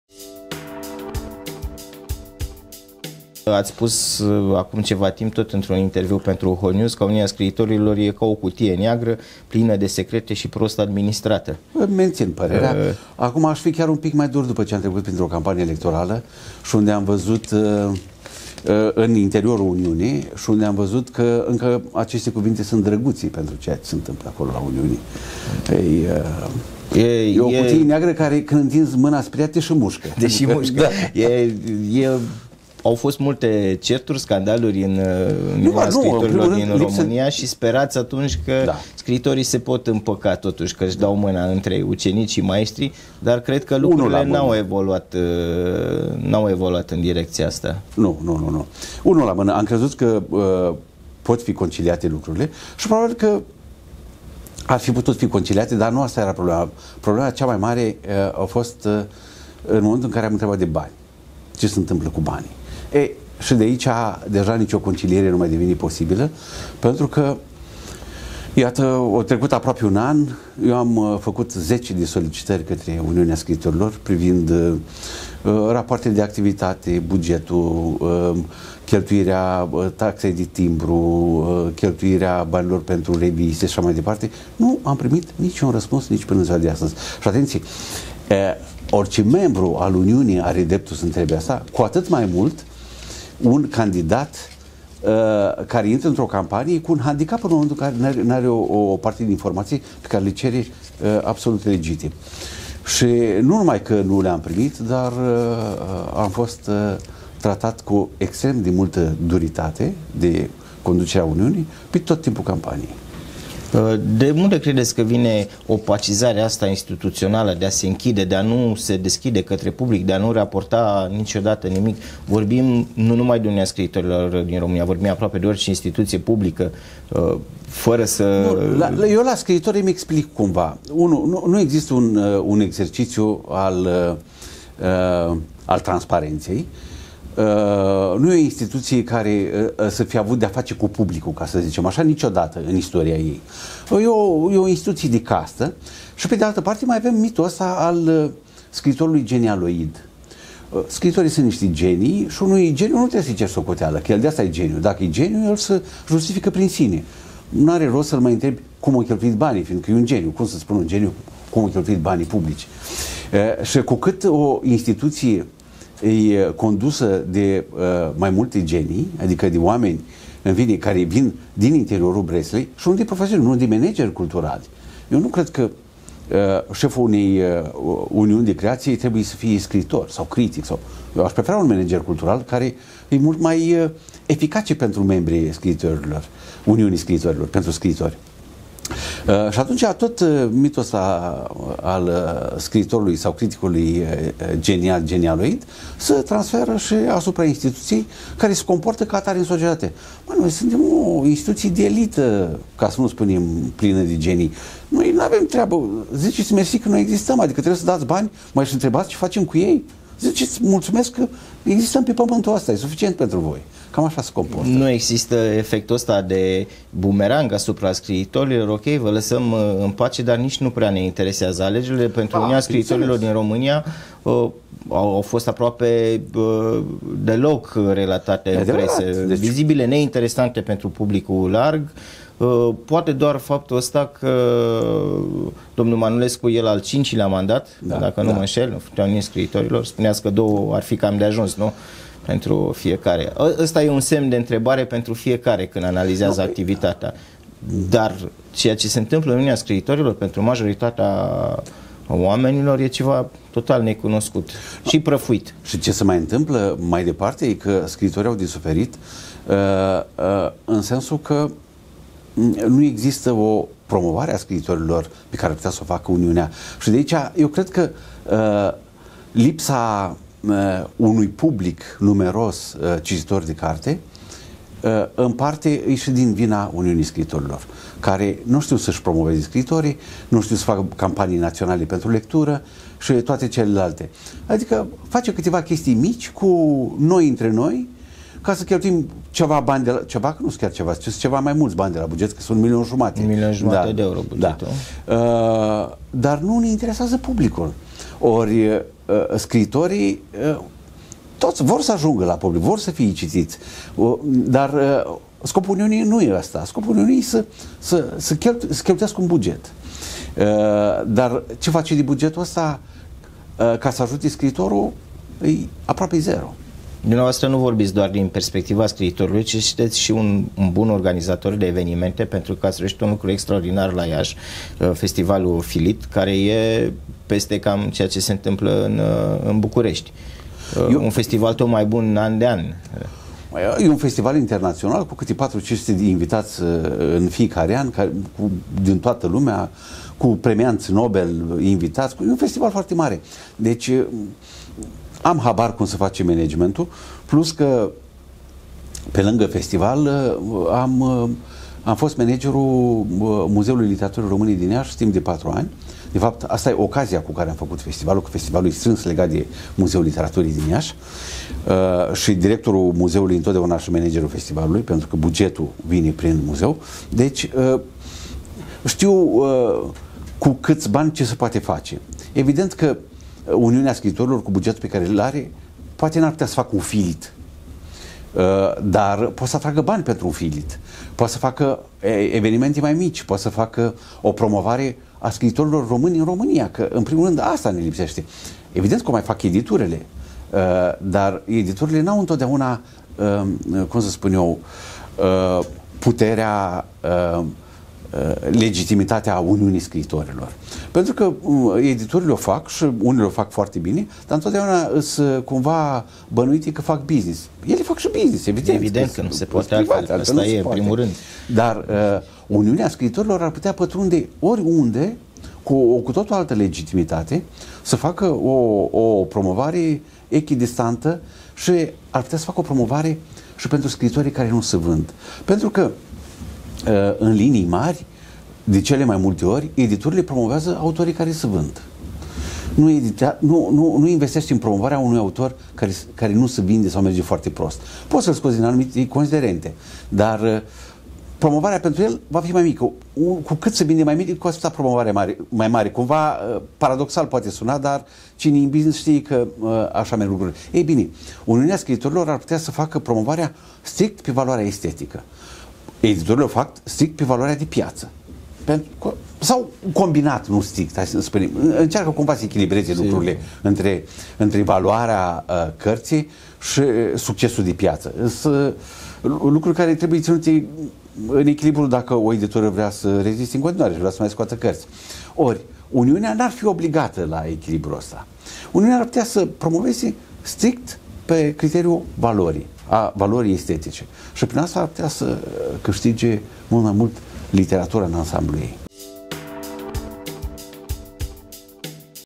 Dan Lungu despre Uniunea scriitorilor si FILIT
Citeste Hotnews.ro
Interviu realizat de Ionut Baias
Imagine si montaj Alexandru Radulescu